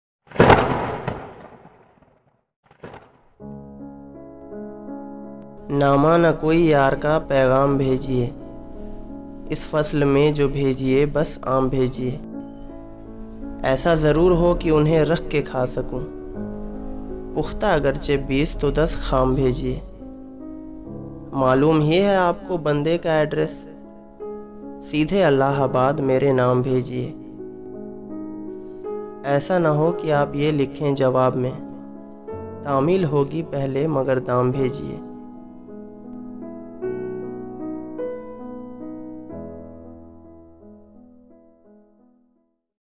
Aam-Naama. A Funny poetry by Akbar Allahabadi
An extremely funny poetry/letter of Akbar Allahabadi.
He also was fond of Mango and he writes to a friend of his to send him some mango through a letter in a poetic way.